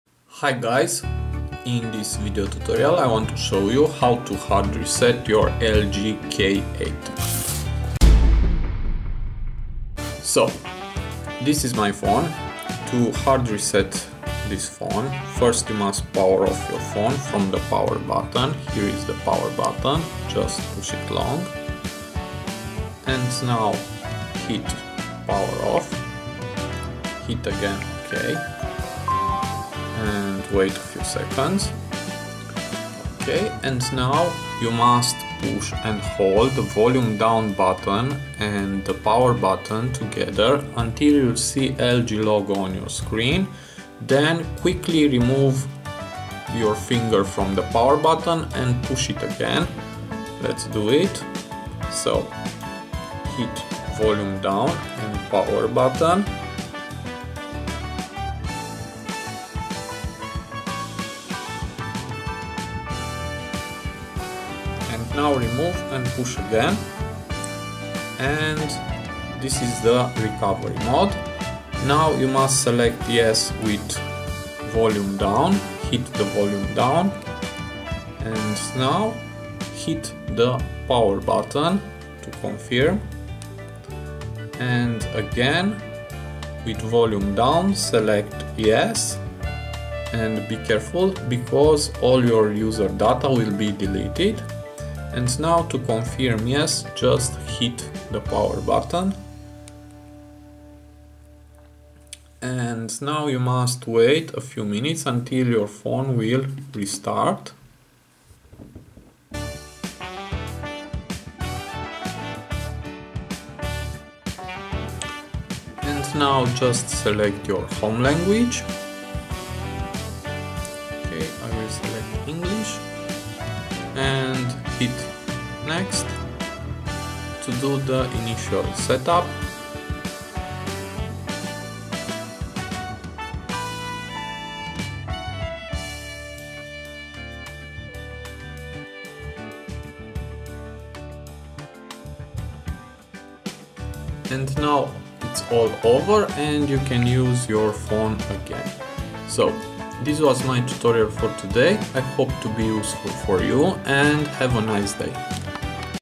How to HARD RESET LG K7, K8, K10
You can also try dr.fone - Android Lock Screen Removal to bypass pattern, PIN, password & fingerprints for Android devices with NO DATA LOSS
In this tutorial I will show you how to hard reset LG K7, K8, K10 (Android 5.1.1 or  6.0.1).You can use this operation to fix issues like freezing, running slow, to remove a file or virus or to clear the memory space or if you forget your security code, password, pattern or pin code. . .
IMPORTANT
Before performing a hard reset, make sure you know your Google login and password. If you have Android version 5.1 or higher on your device and you have added a Google account, you might be asked to input the Google account login and password after you perform o software repair.If you cannot provide this information during the setup process, you won't be able to use the device at all after software repair!

Warning! You do this at your own risk! Make sure your phone battery is fully charged!
If you reset or update your phone, all your data may be lost, so it's good to do a full backup before!

This method will not upgrade or downgrade the firmware version of your LG and it will NOT UNLOCK the network lock!
This method does not reset the PIN code of the SIM card! If your phone has been ROOTED this method will NOT UNISTALL the ROOT.

This method was tested on LG K8 4G K350n but works also on:
- LG K8 - LG Phoenix 2 (AT&T)
- LG K7 X210 (Europe); X210DS (Russia); MS330 (MetroPCS); LG Tribute 5 LS675 (Boost Mobile)
- LG K10 K420N (Europe); K430DS (Russia); K430DSF (LATAM); K430DSY (Taiwan)